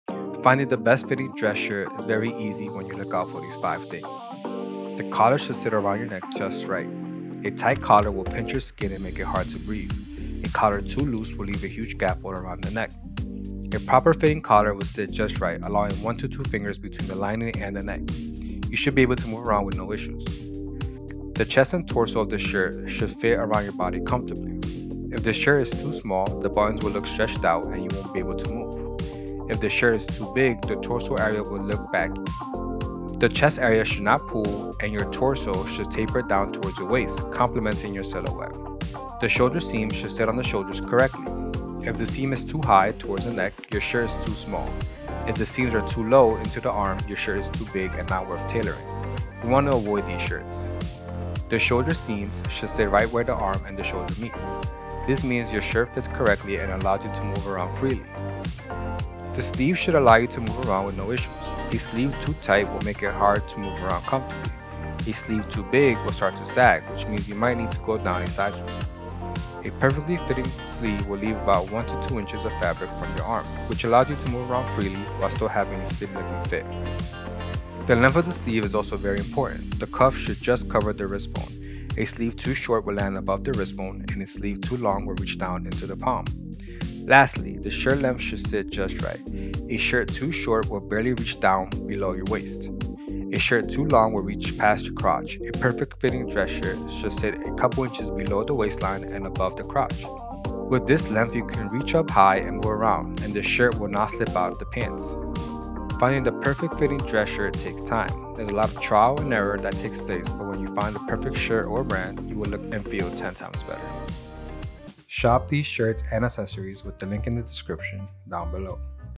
How Should a Dress Shirt Fit | Proper Fitted Collar & Sleeves
Are you unsure about how a dress shirt should fit? In this comprehensive guide, we'll break down the essential aspects of dress shirt fit, including dress shirt sizes, measurements, collar fit, sleeve length, and neck size. Whether you're dressing for a formal event or refining your daily office attire, nailing the fit is the first step towards looking and feeling your best in perfectly fitted dress shirt.

In this video, we'll discuss:

👔 Key considerations for dress shirt sizing and measurements.
📏 How to determine the ideal sleeve length and collar size.
✂️ Tips and tricks for achieving a tailored look without a tailor.

Say goodbye to uncomfortable or ill-fitting dress shirts! Watch our tutorial to master the art of dressing sharp with perfectly fitting dress shirts.

Dressing for success starts with understanding how a dress shirt should fit.

Here’s our comprehensive guide on How Should a Dress Shirt Fit